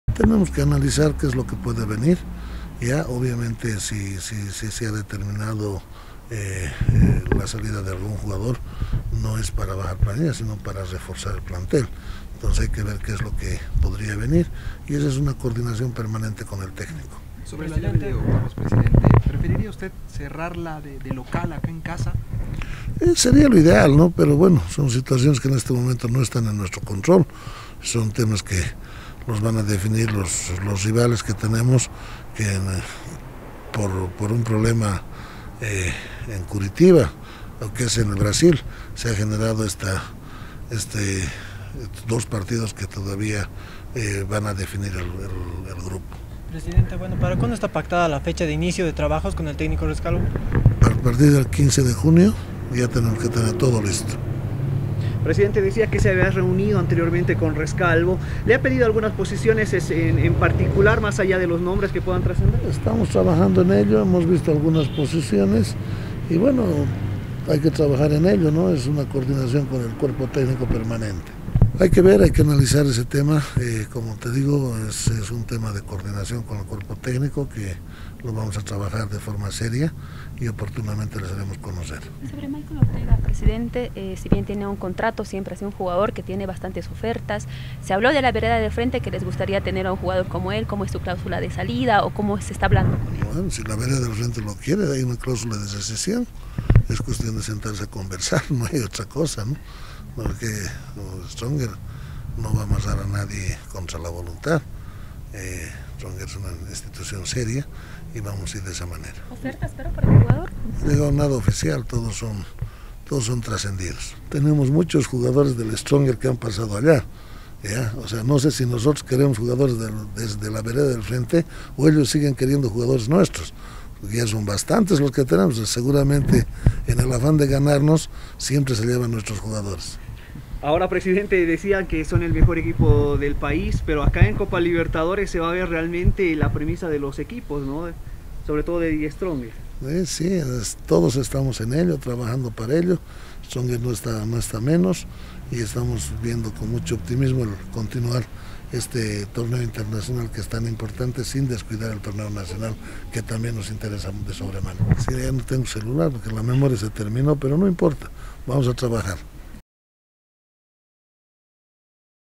HAY QUE REFORZAR EL EQUIPO
Esto es lo que dijo el presidente de TheStrongest acerca de las posibilidades de refuerzos para los octavos en la Libertadores. Seguinos en todas nuestras señales: ERBOL 100.9 FM para LaPaz y ElAlto y emisoras en el interior. Además dale click a nuestros enlaces